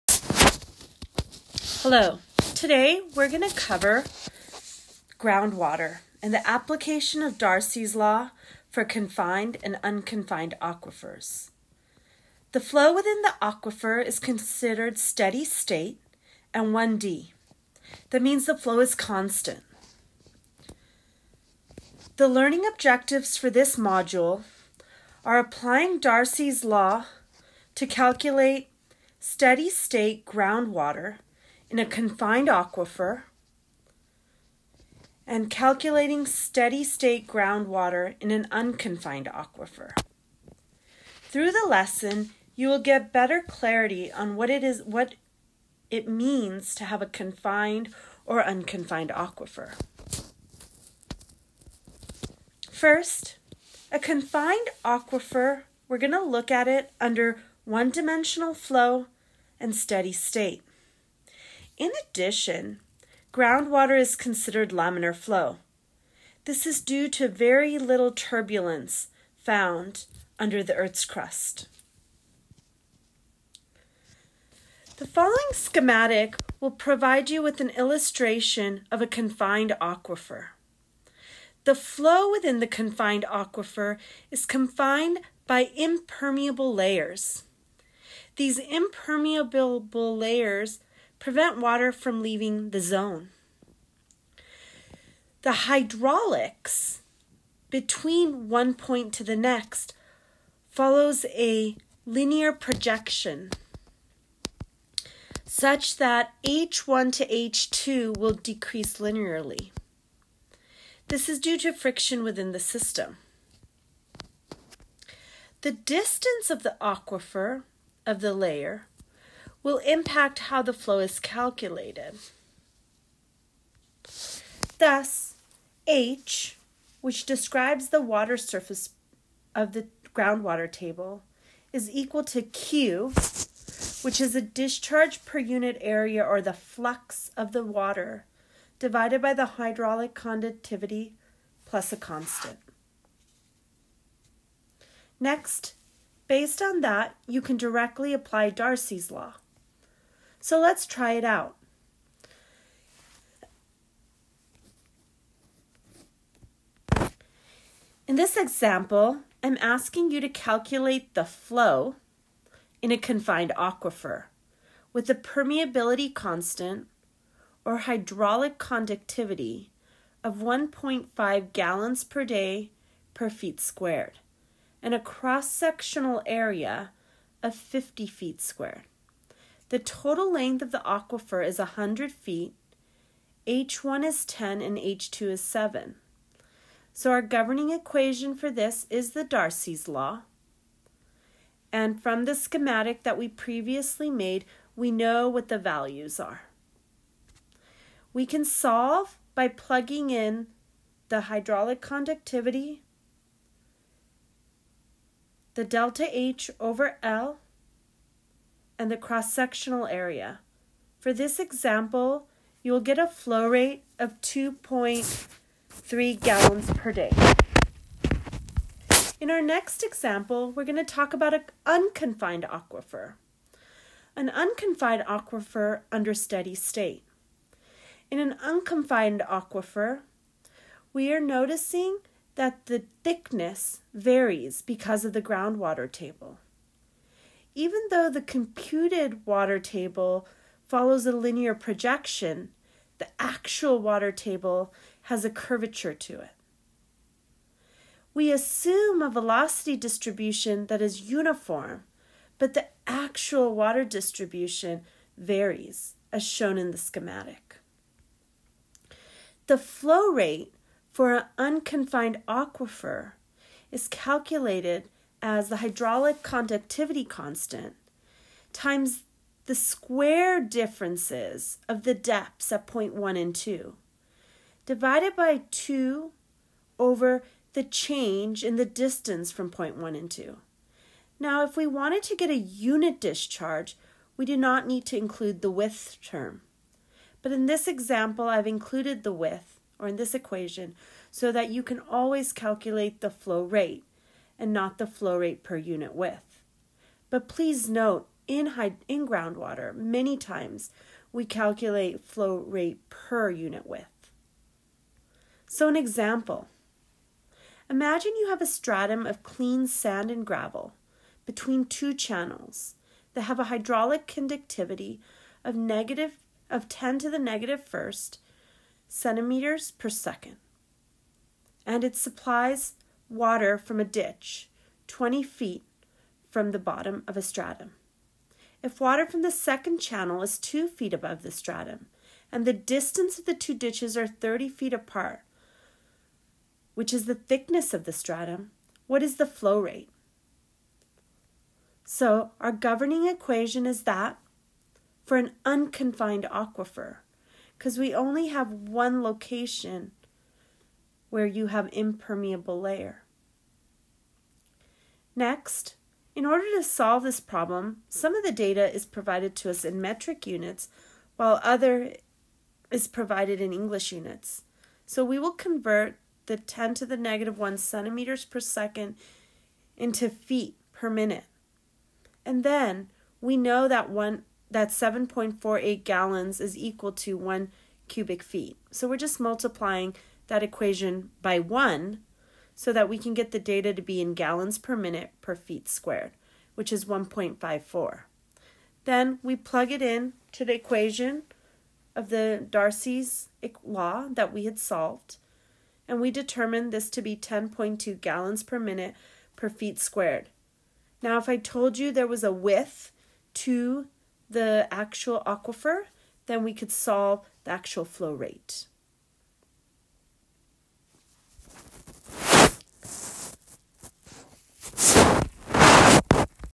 SteadyStateGroundwater
The following video covers the basic concepts of groundwater flow for steady state 1-D systems.